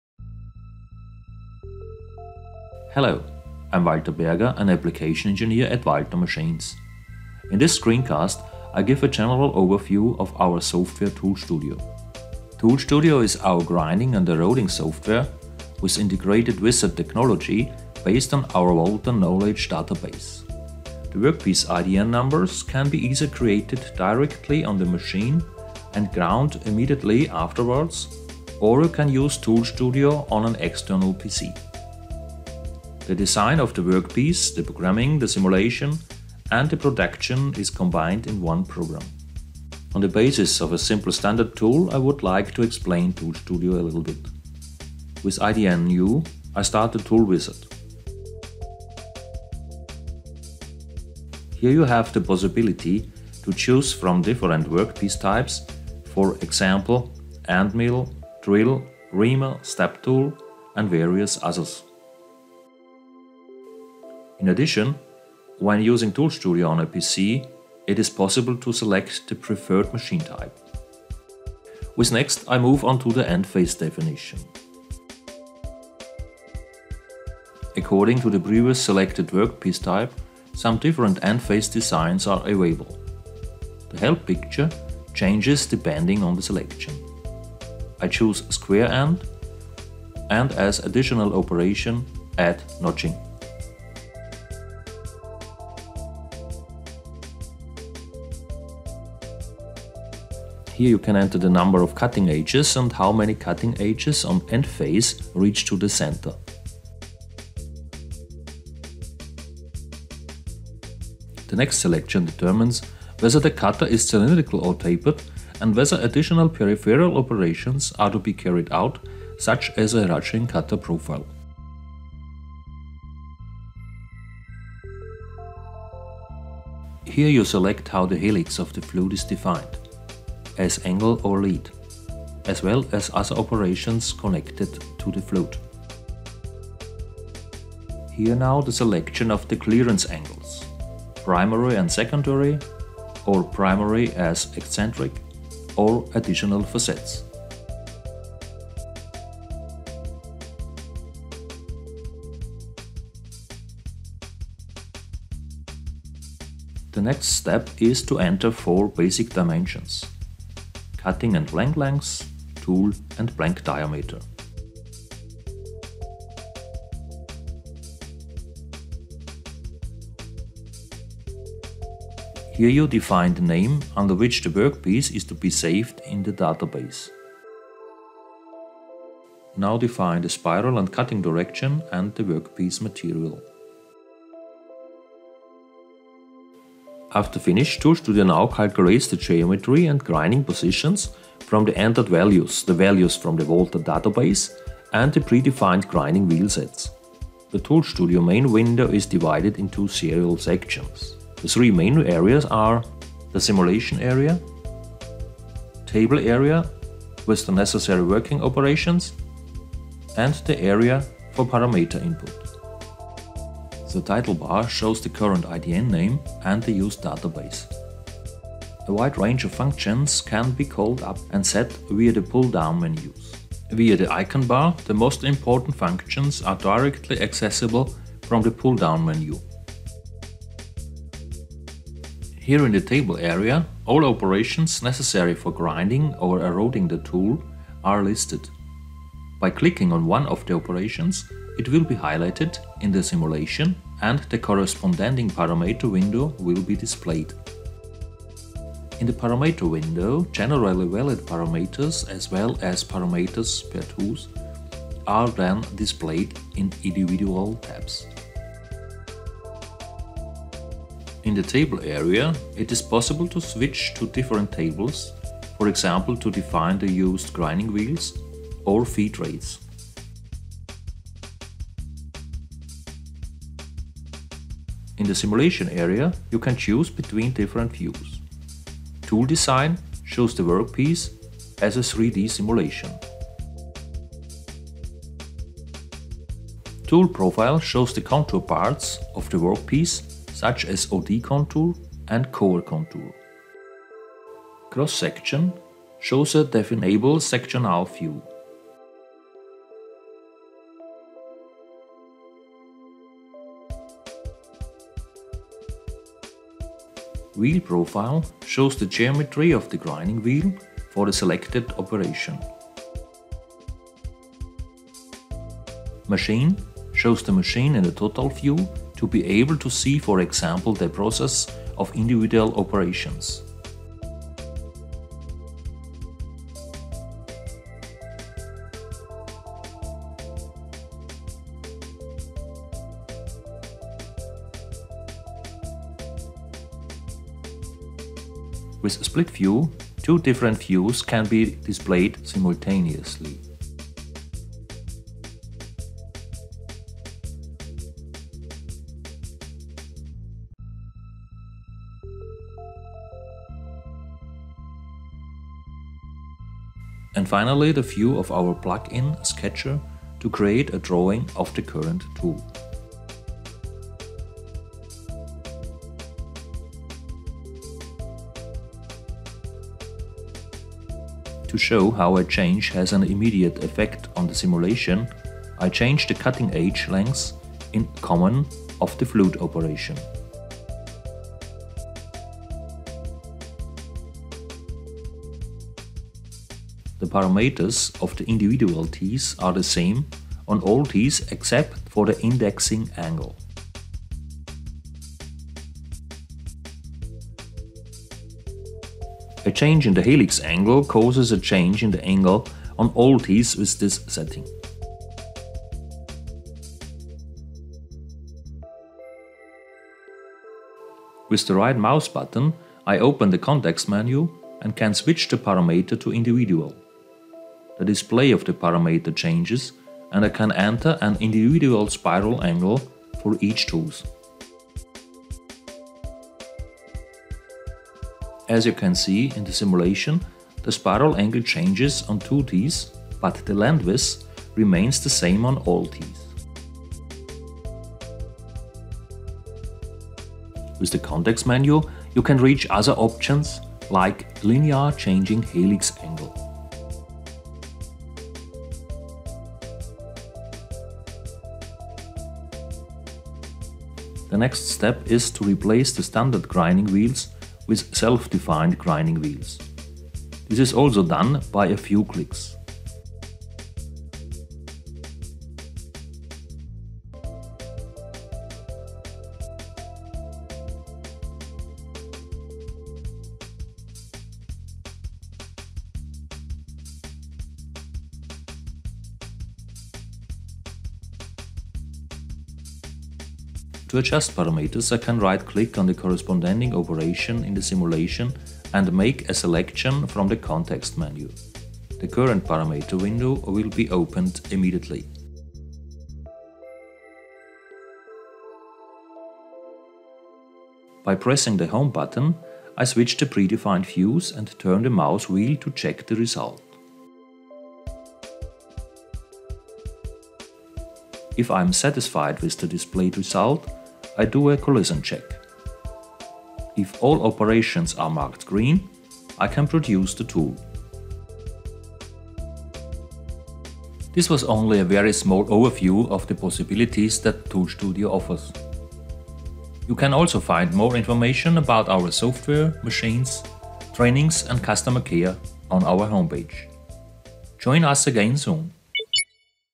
HELITRONIC TOOL STUDIO
Our grinding software HELITRONIC TOOL STUDIO helps to turn the user into a grinding professional! It allows the editing of all tool parameters from design, programming and simulation to manufacturing. All manufacturing steps of a tool are combined in this software application and enable the fast solution of any manufacturing task.